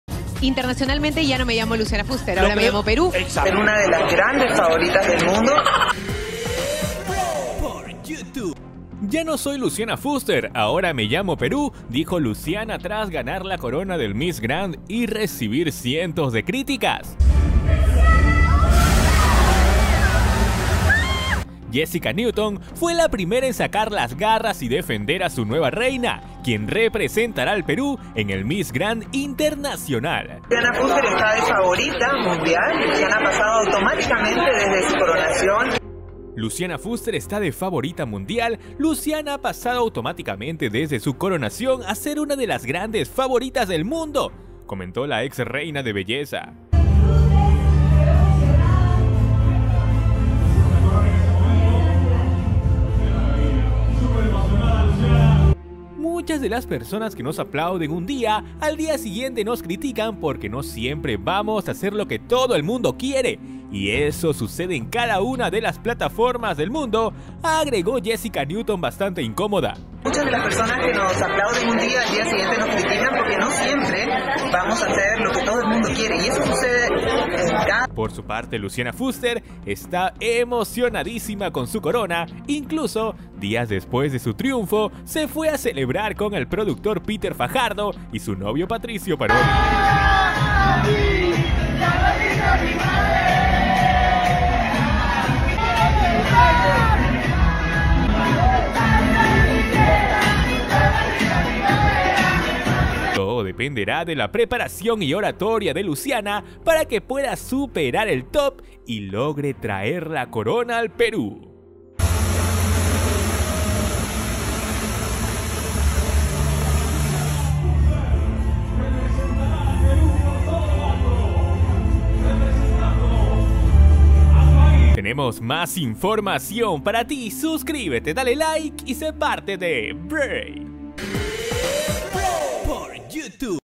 JESSICA NEWTON JURA QUE LUCIANA ES LA FAVORITA DEL MUNDO Y LA DEFIENDE
Jessica Newton decidió defender a su nueva reina Luciana Fuster tras ganar el Miss Grand Perú 2023.